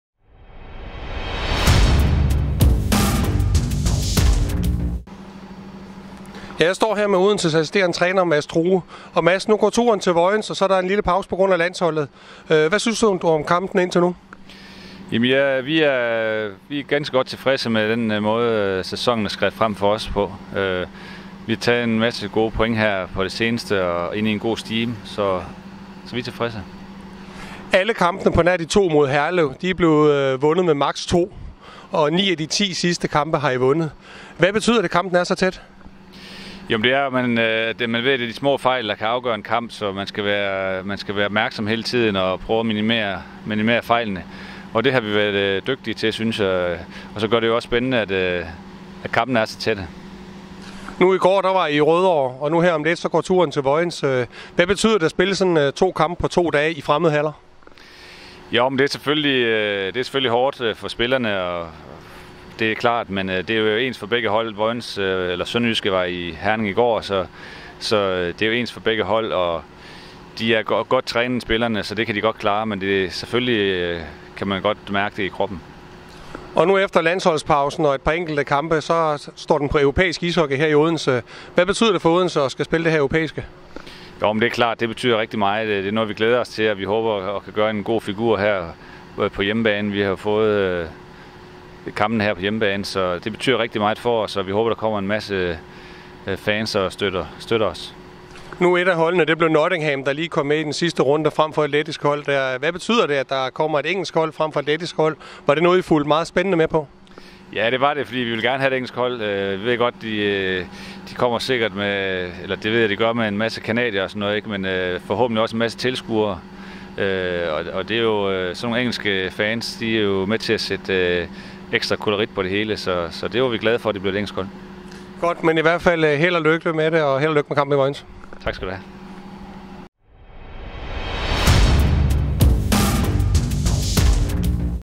Interview med Odense Bulldogs assistenttræner Mads True
Interview med Odense Bulldogs assistenttræner Mads True, inden kampen mod Sønderjyske